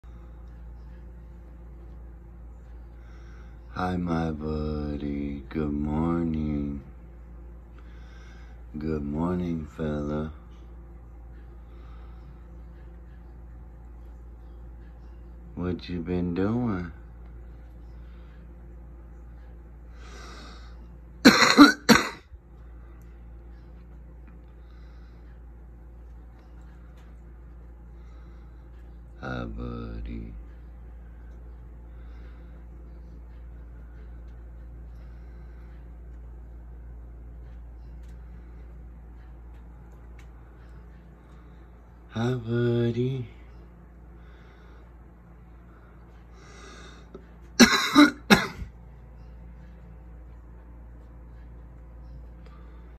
chameleon trust (chameleon morning🌞)
chameleon trust (a chameleons morning)waking up🌞.He is shedding.